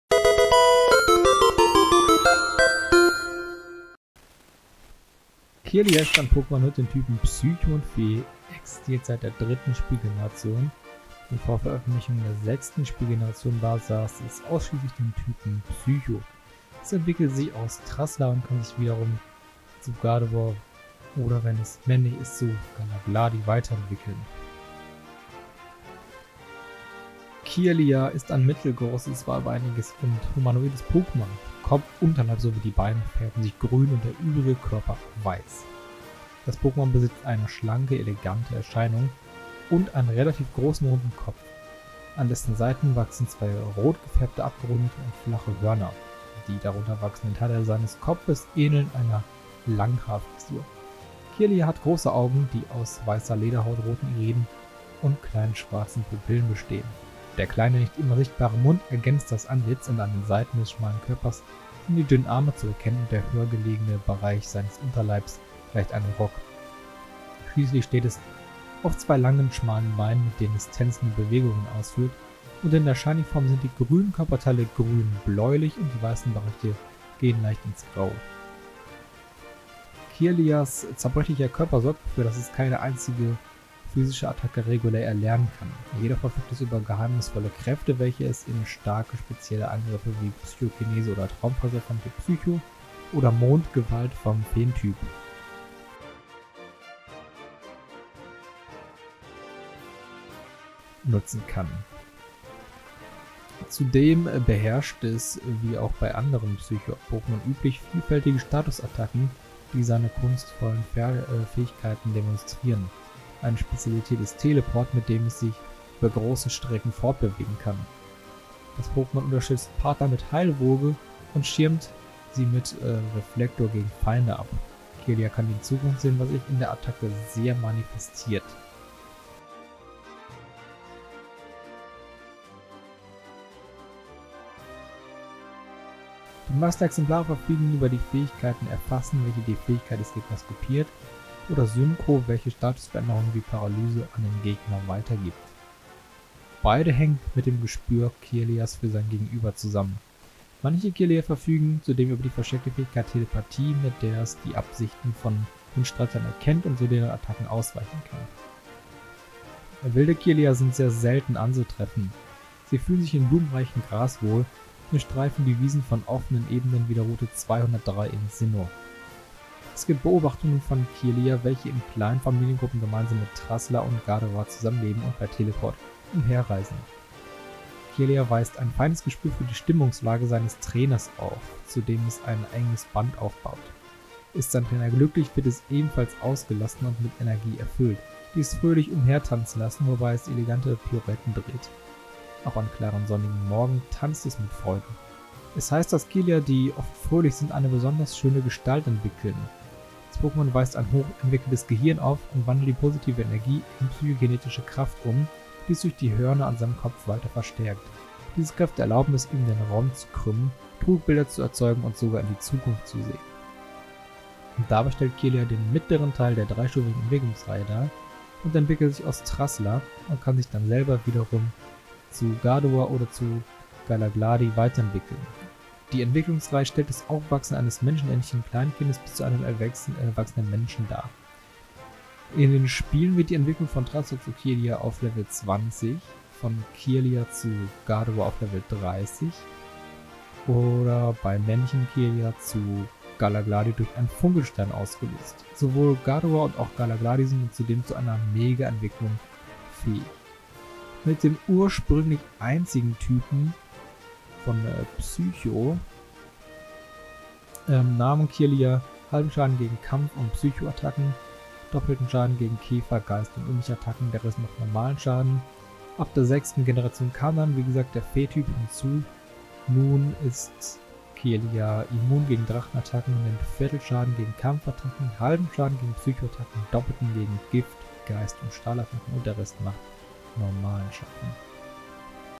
Das Pokémon #281 | Kirlia
▶️Worum gehts in diesem Video:

Ich stelle euch das Pokémon Kirlia vor. Dabei schauen wir auf das Aussehen, Attacken, Verhalten, Lebensraum, Entwicklung und Typ Schwächen. Viel Spaß :D

0:00 Intro
0:27 Aussehen
1:19 Attacken & Fähigkeiten
2:38 Verhalten & Lebensraum
3:33 Entwicklung
4:10 Typ Schwächen